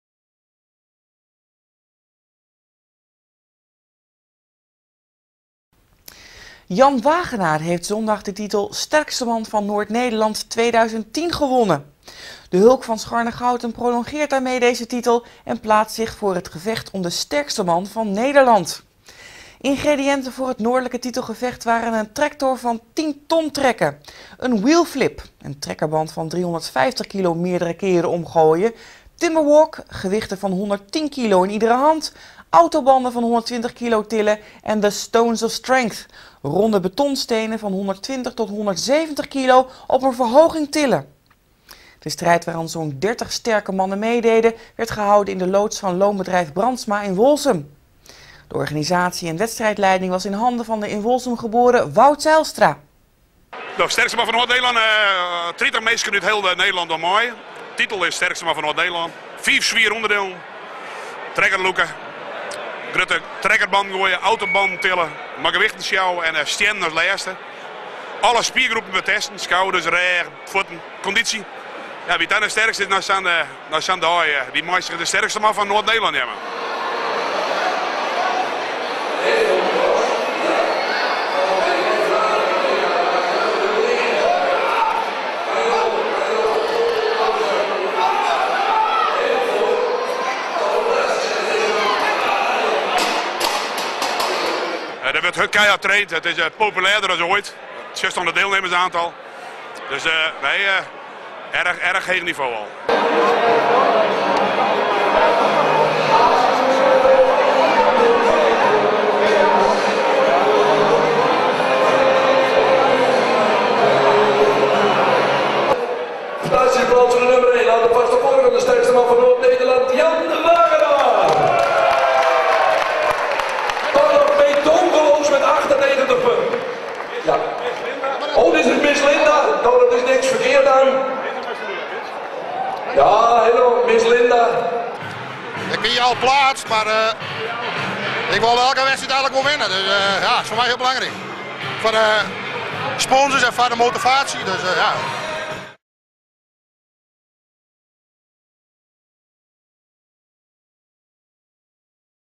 Sterkste man van Noord-Nederland 2010.
deelnemers:
Jitse Kramer.
Kelvin de Ruiter..
Johan Langhorst.
Jan Wagenaar.
Arno Louwsma.

En anderen ??
Verslag GP-TV.
Foto’s Doetze Hovius.
Organisatie Wout Zijlstra.